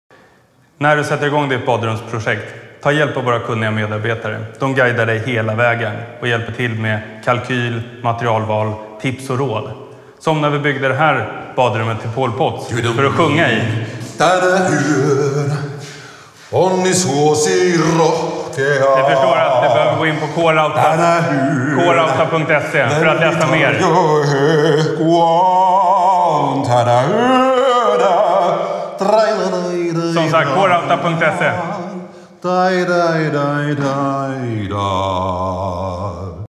K-rauta - Tips och råd: badrum
Hur ska man egentligen tänka när man förnya badrummet? C-M delar med sig av lite tankar och Jarmo ”hjälper till”.